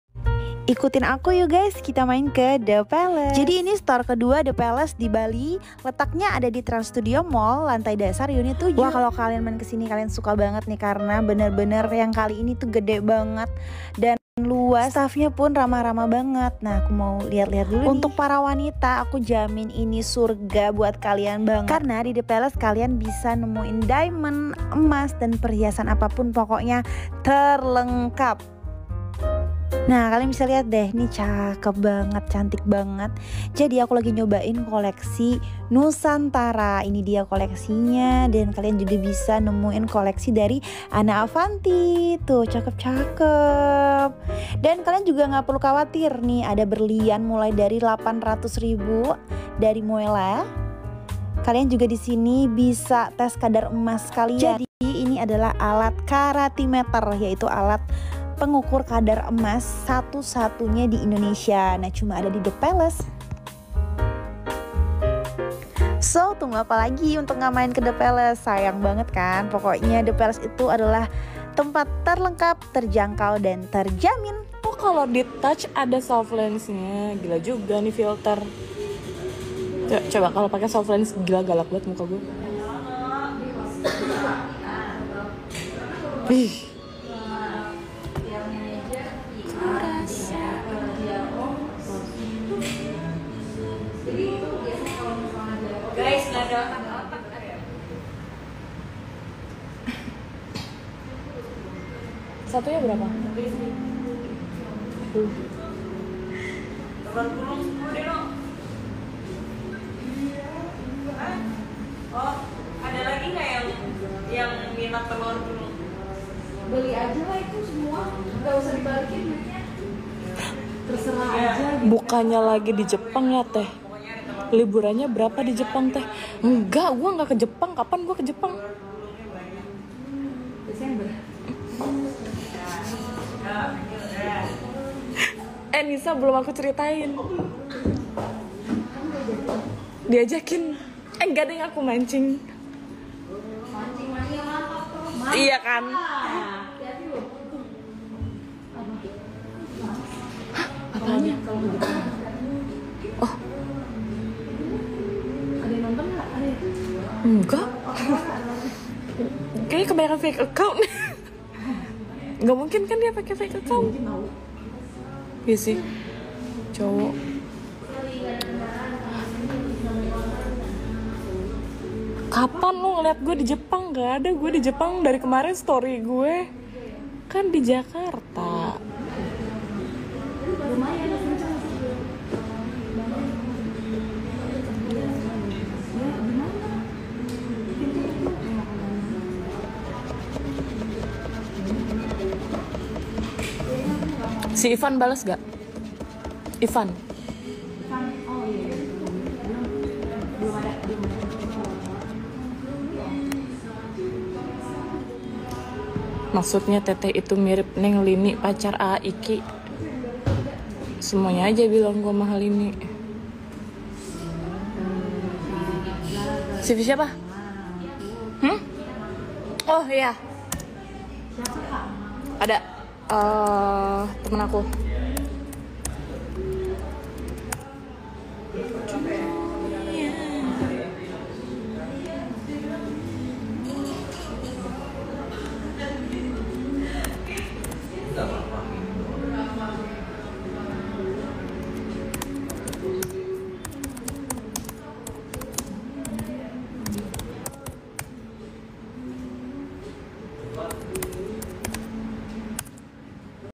Wow..😲 Kembaran Mahalini jadi Bintang Iklan Berlian The Palace di Trans Studio Mall Bali (Dr Nindy)
The Palace toko berlian terlengkap, terjangkau, dan terjamin kualitas dan kuantitasnya, kini telah hadir di Trans Studio Mall Bali.